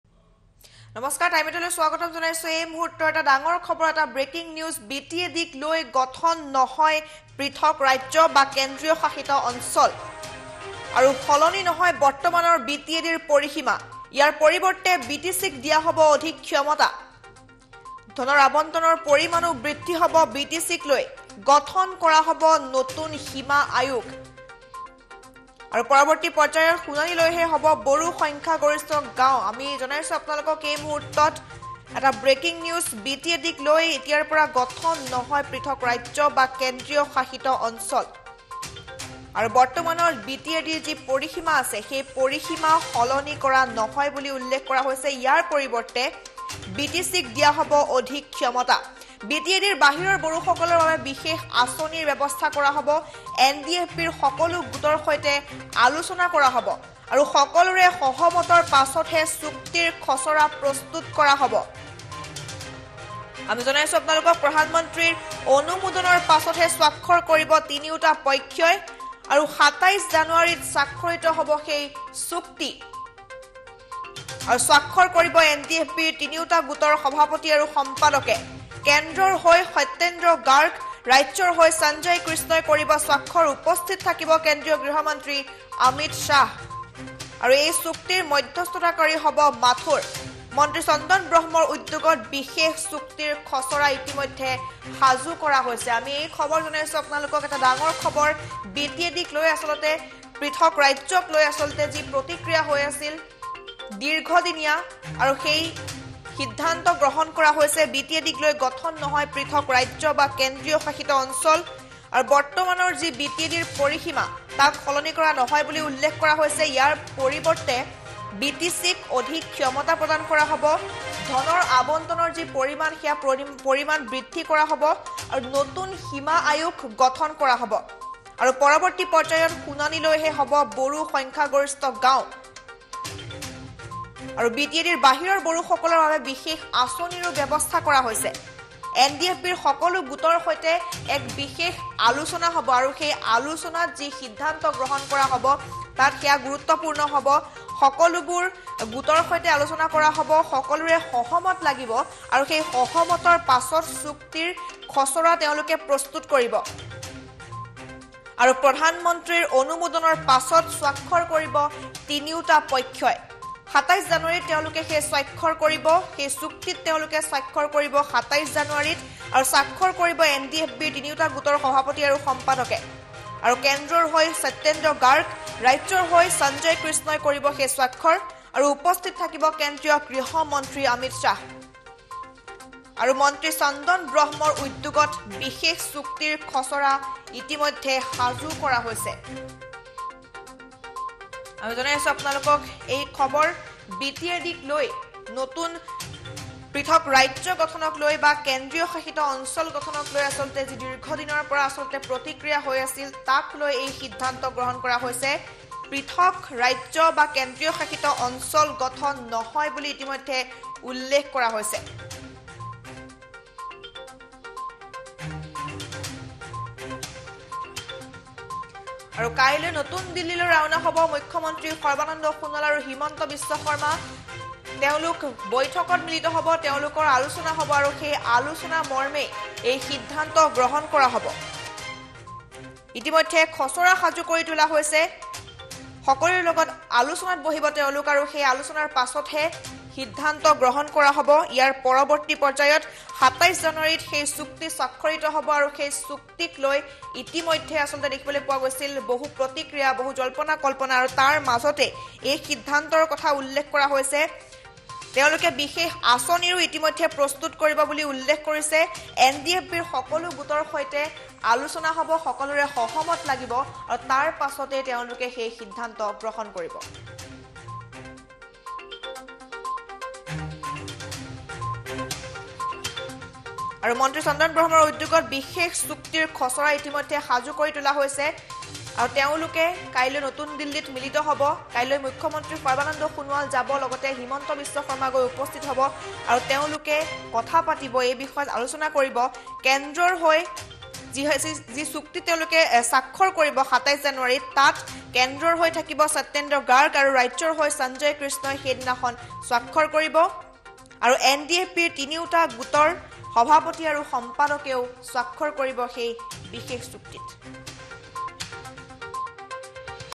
BIG_BREAKING BTADক লৈ গঠন নহয় পৃথক ৰাজ্য বা কেন্দ্ৰীয় শাসিত অঞ্চল
BTADক লৈ গঠন নহয় পৃথক ৰাজ্য বা কেন্দ্ৰীয় শাসিত অঞ্চল, সলনি নহয় বৰ্তমানৰ BTADৰ পৰিসীমা